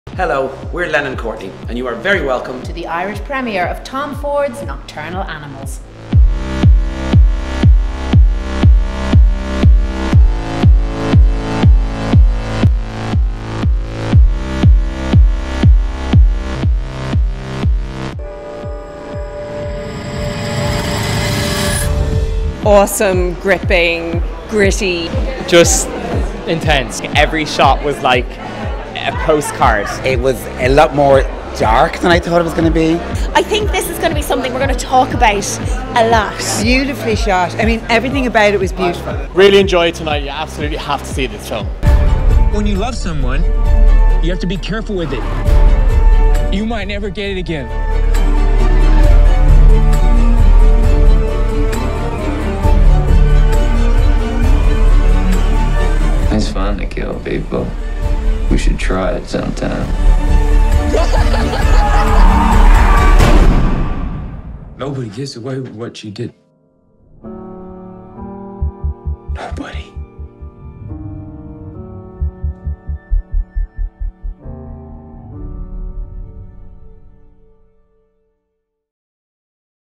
Nocturnal Animals Irish Premiere Hosted by Lennon Courtney
In cinemas Friday.

Cast: Amy Adams, Jake Gyllenhaal, Michael Shannon, Aaron Taylor-Johnson
Directed by:  Tom Ford
Written by:                        Tom Ford
Produced by:   Tom Ford, Rob Salerno

From writer/director Tom Ford comes a haunting romantic thriller of shocking intimacy and gripping tension that explores the thin lines between love and cruelty, and revenge and redemption. Academy Award nominees Amy Adams and Jake Gyllenhaal star as a divorced couple discovering dark truths about each other and themselves in NOCTURNAL ANIMALS.

The film will be released by Universal Pictures on November 4th